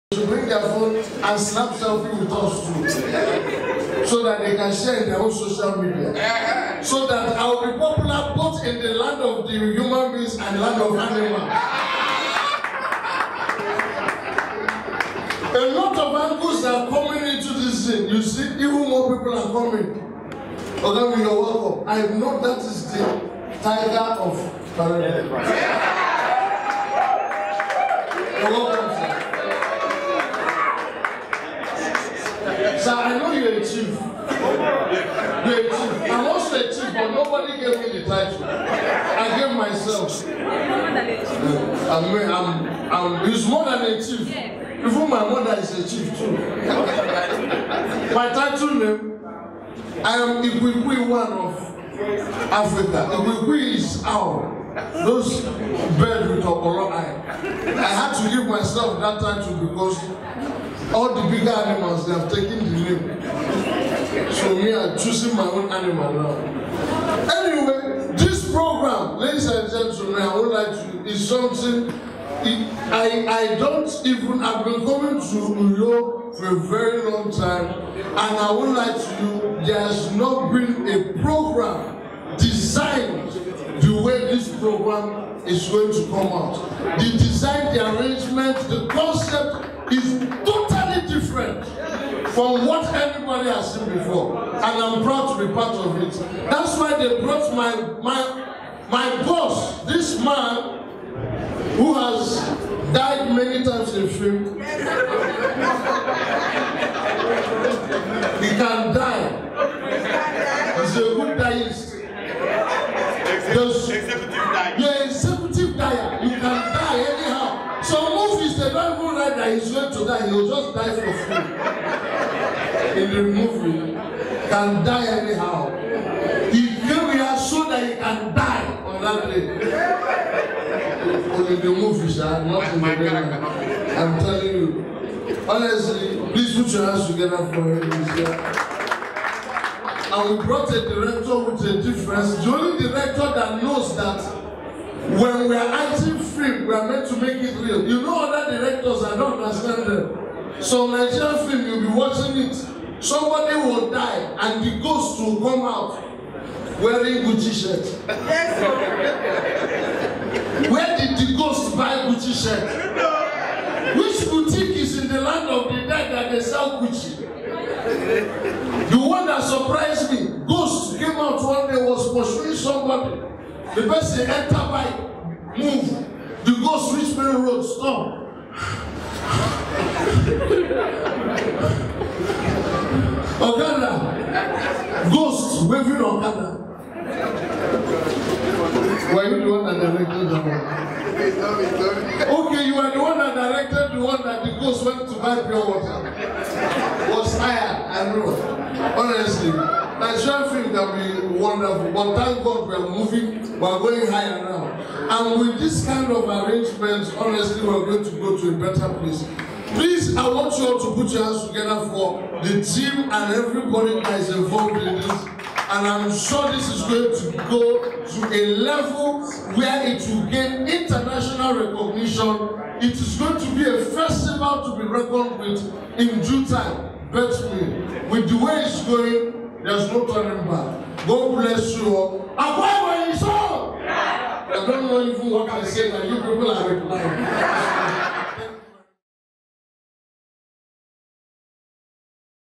KLINT DA DRUNK @ COLOURS OF MKPAT ENIN ON ROCKLAND HOUSE TV
KLINT DA DRUNK was at the COLOURS OF MKPAT ENIN held in AKWA IBOM STATES and documented by ROCKLAND HOUSE TV.